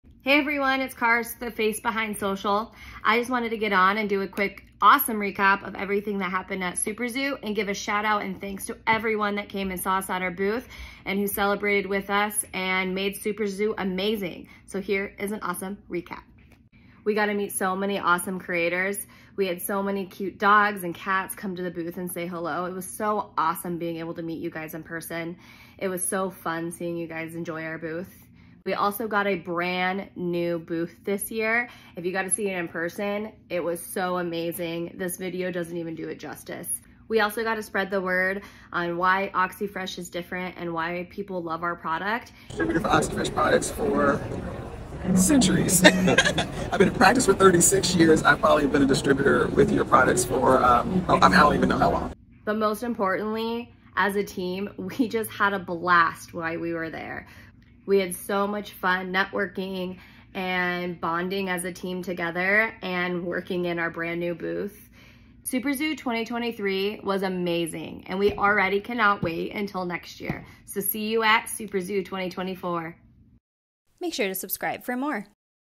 SuperZoo 2023
SuperZoo, you've left us buzzing with excitement! Our brand new booth was the place to be, where we loved connecting with creators, retailers, and distributors.

But it wasn't just business – it was a blast! We soaked up every moment of team bonding and shared laughter, jokes and amazing memories.

Huge thank you to everyone who swung by to learn what sets Oxyfresh apart. Your enthusiasm fuels our passion!

And a standing ovation for our unstoppable team – Tom, Melissa, Craig, Susan, and Kars – your hard work made this show an absolute triumph.